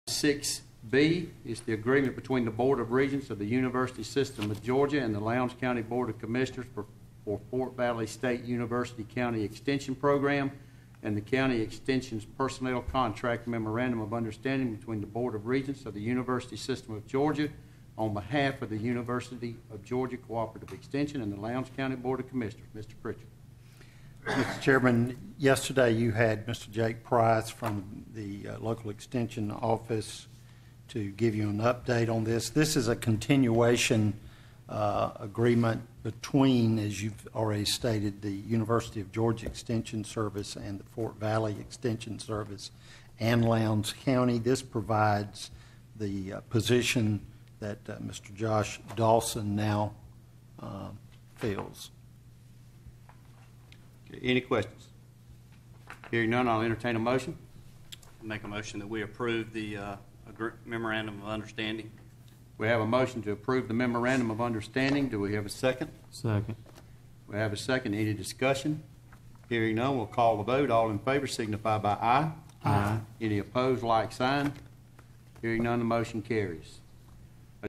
04387
Lowndes County Board of Commissioners (LCC) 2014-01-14 Regular Session